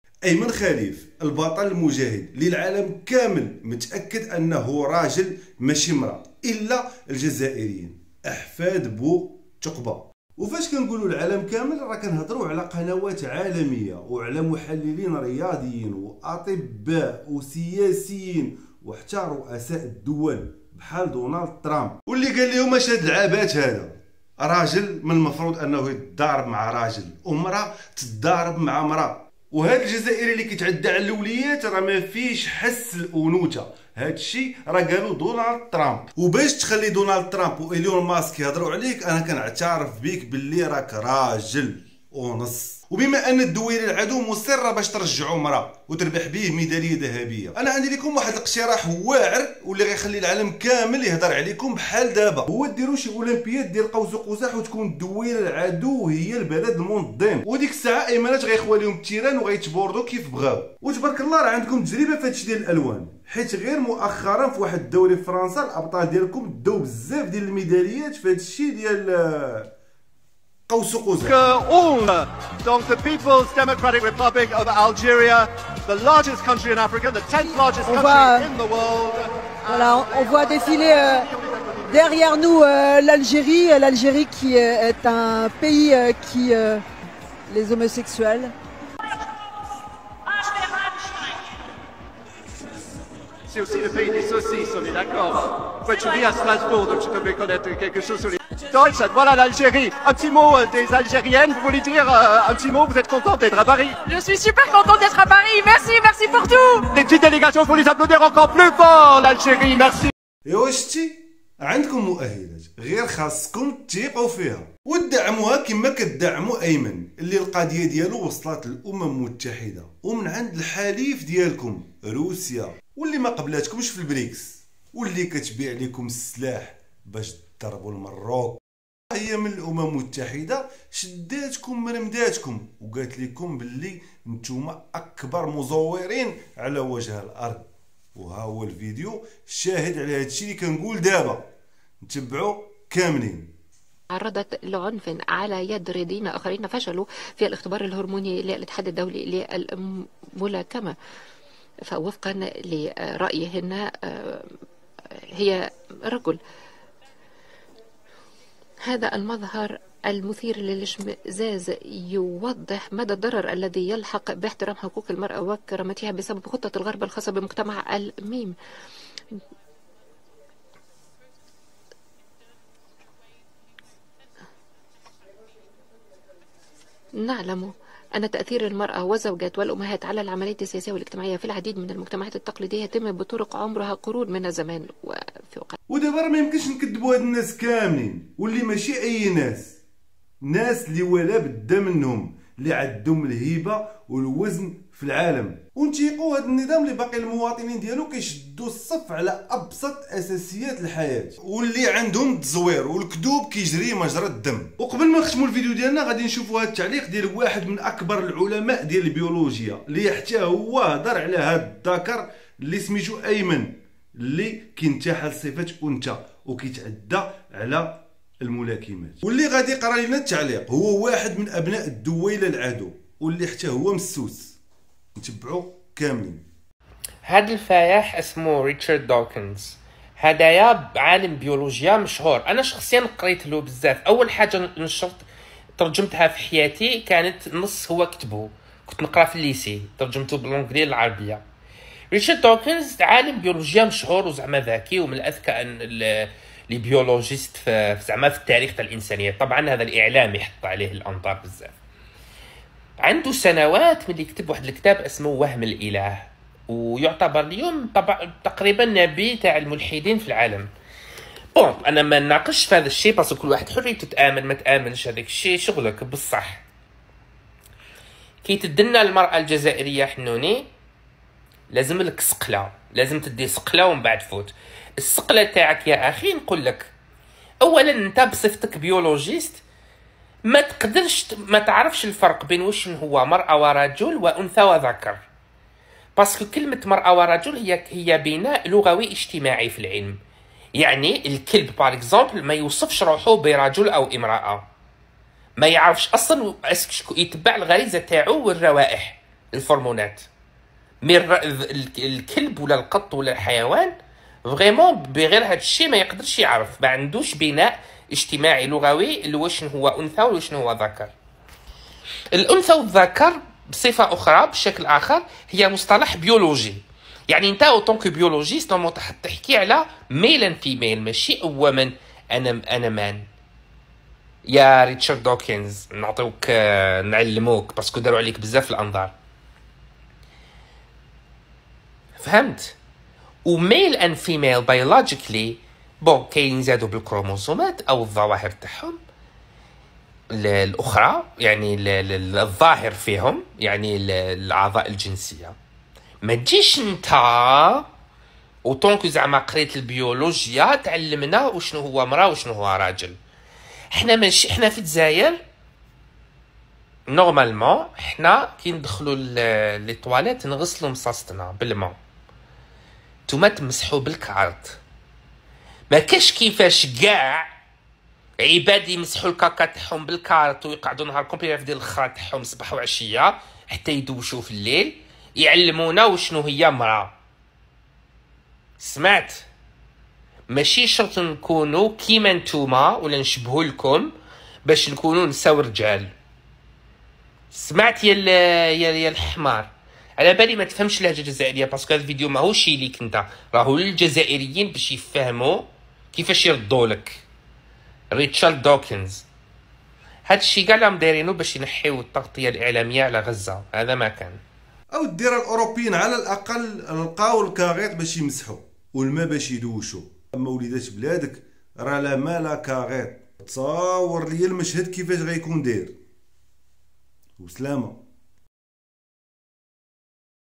ممثل روسيا في الامم المتحدة: ايمان خليف رجل /دونالد ترامب/ايمان رجل /عالم بيولوجيا ايمان رجل 😂😂
مندوب روسيا 🇷🇺 في الأمم المتحدة يفضح حقيقة المتحول الجزائري 🇩🇿 ايمان خليف الذي يشارك في منافسة للنساء في أولمبياد باريس و ذالك بعدما تم طرده من طرف الاتحاد الدولي للملاكمة بعد التأكد بواسطة الاختبارات الطبية بأنه ذكر و ليس انثى .
الذي أضحكني أكثر في الموضوع هو المندوب الجزائري الذي يتمتم مثل الفاسقة أمام المندوب الروسي